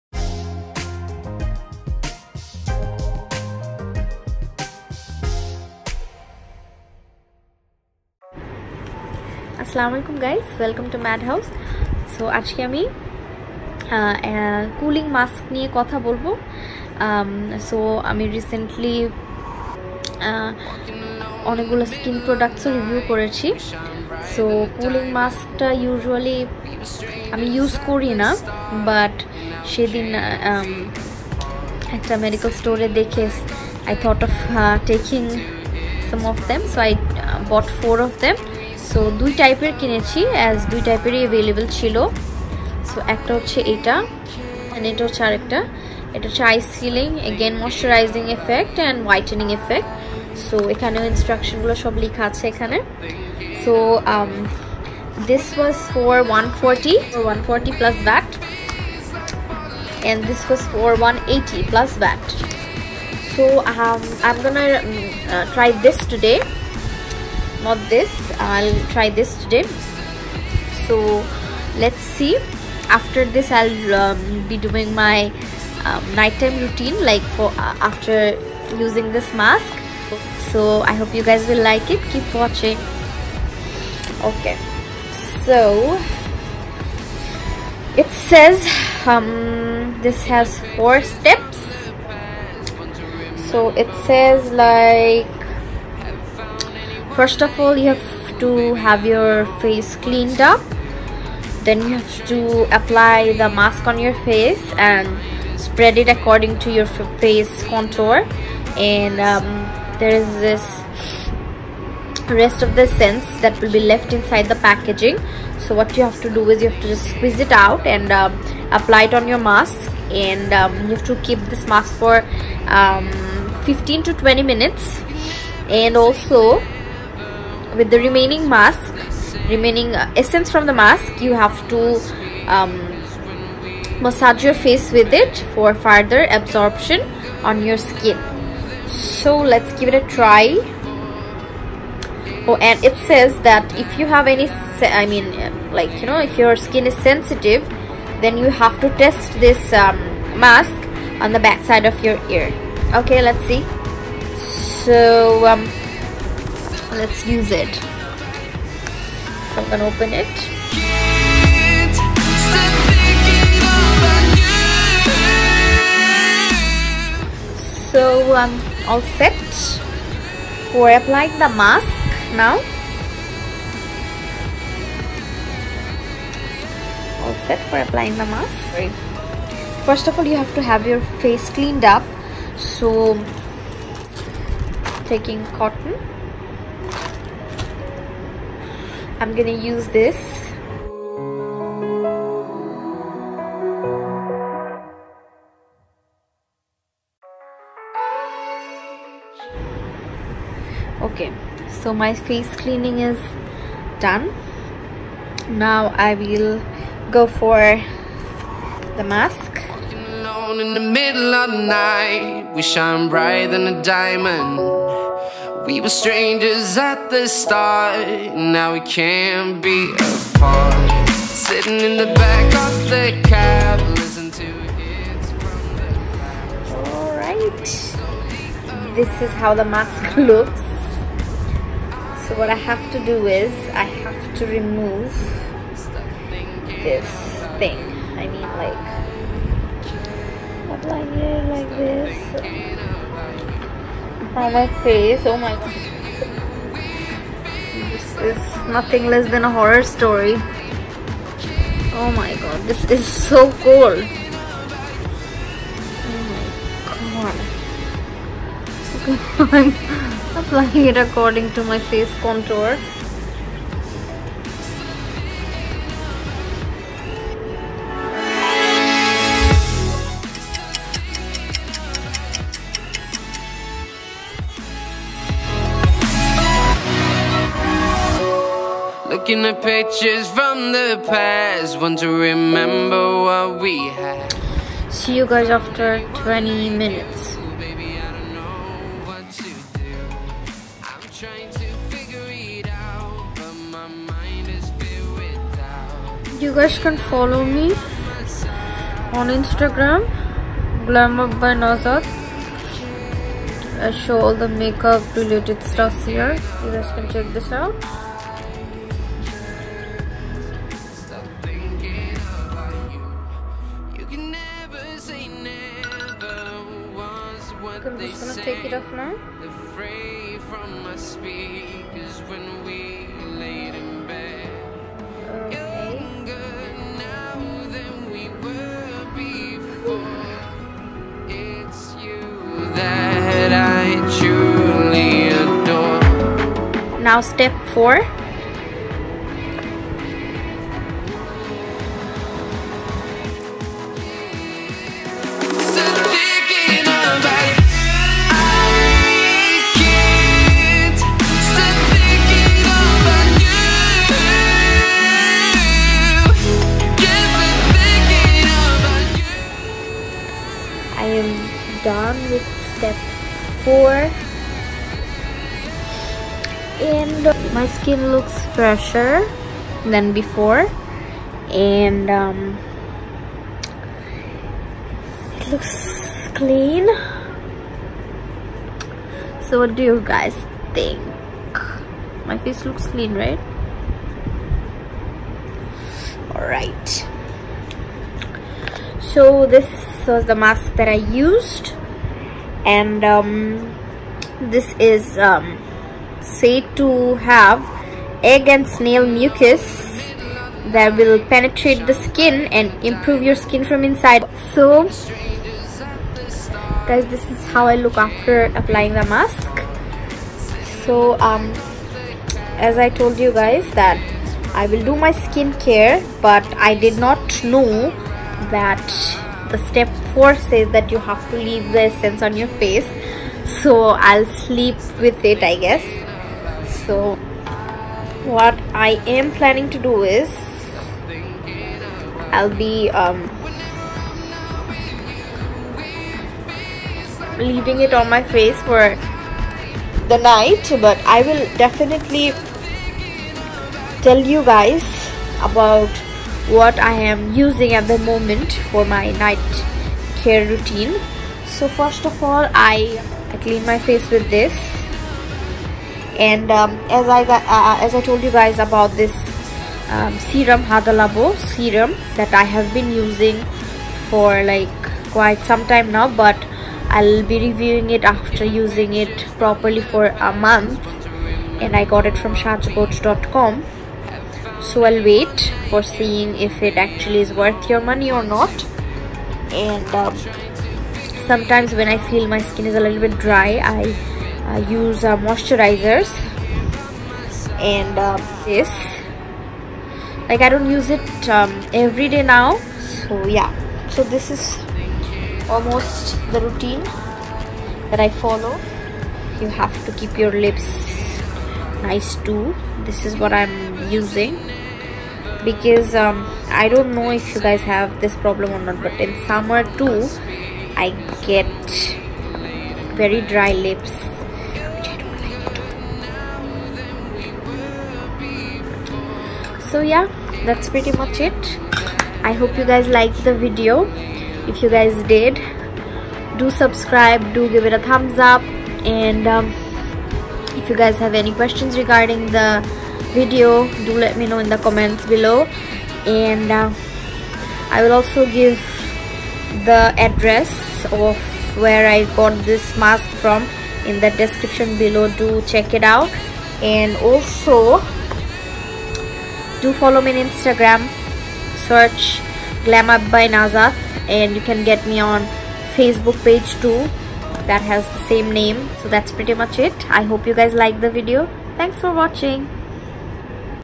EGG AND SNAIL MUCUS MASK! |MAD HOUSE| Gross Or Effective?
this mask that i have used contains egg and snail mucus , do test at the back of your ear, if you have a sensitive skin. blue mask costed me 140 bdt + vat and black mask was for 180 bdt + vat. since i have used both , i recommend the black mask. bought it from wellness just beside Unimart dhanmondi.